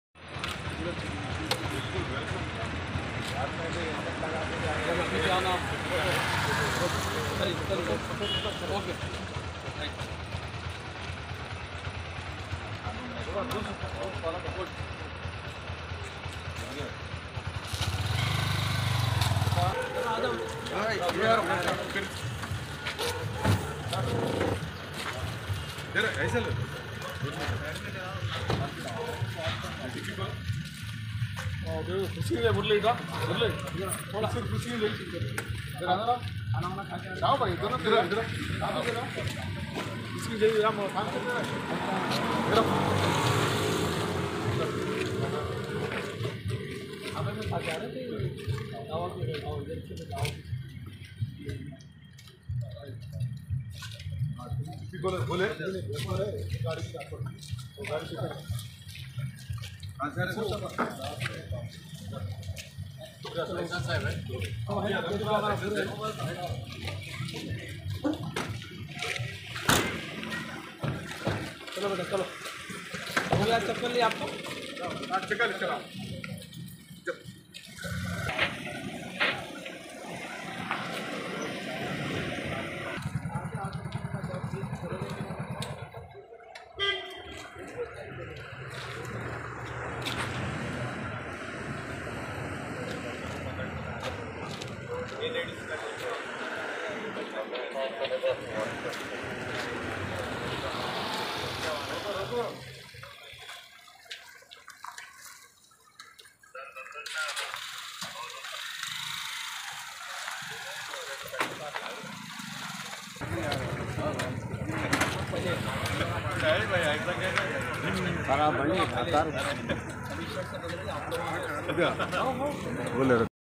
Kal Raat Gadiyoon Ke Talashi Mohim Tadbun EidGah Road Per Kalapathar PS Limits || HYDTAK
Kal Raat Gadiyoon Ke Talashi Mohim Tadbun EidGah Road Per Kalapathar Police station limits..